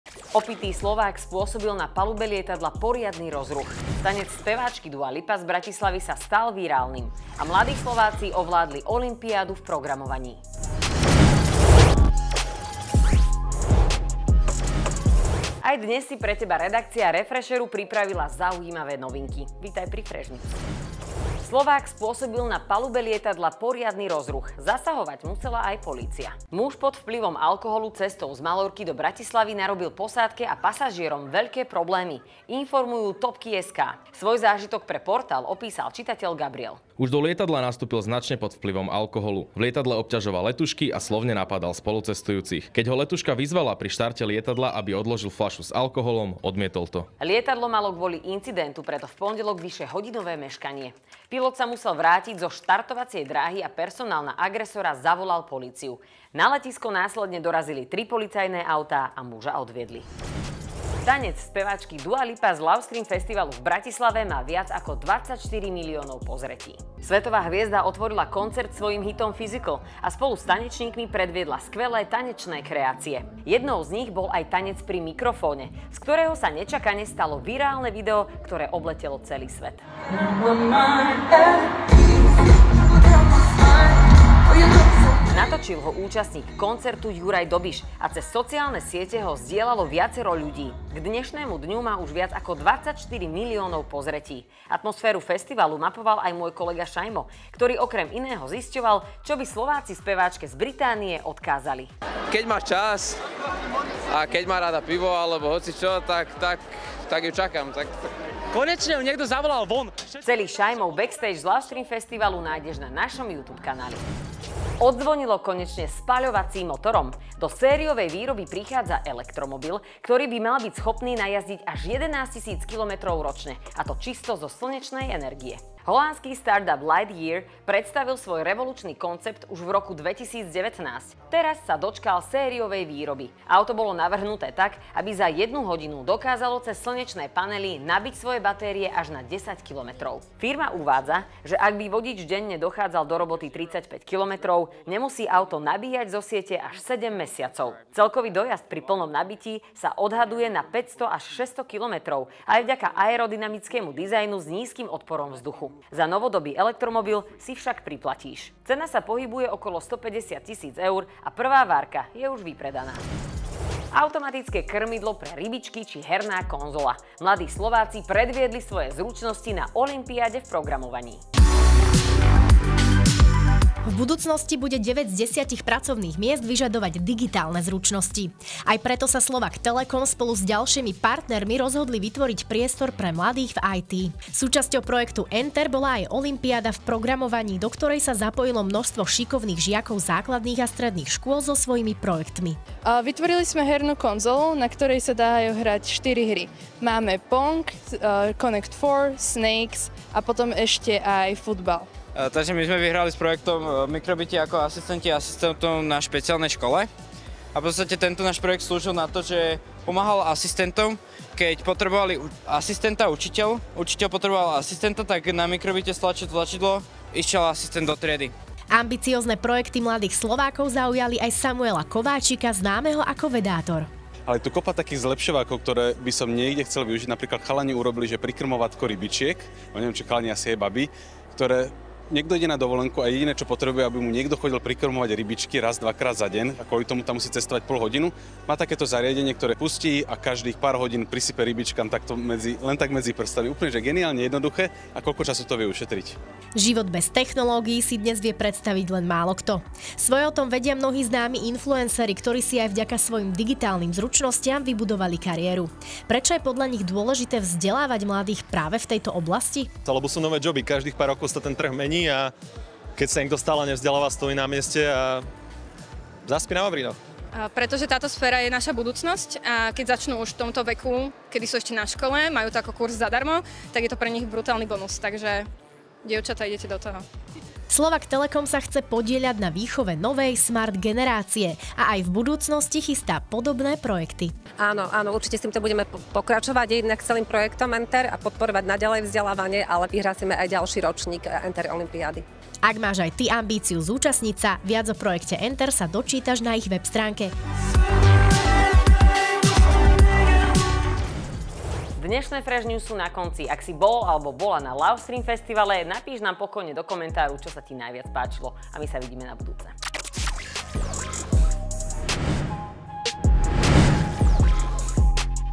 Polonahého Slováka v lietadle musela zobrať polícia. Opitý obťažoval letušky
Slovák pod vplyvom alkoholu cestou z Malorky do Bratislavy narobil posádke a pasažierom veľké problémy. Odmietal sa vzdať fľaše s alkoholom, vulgárne nadával ľuďom v okolí a vyzýval ich na bitku.

KAPITOLY:
0:00 - 0:20 Intro
0:21 - 1:53 Dua Lipa Lovestream
1:54 - 2:42 Elektromobil
2:43 - 5:16 Enter olympiáda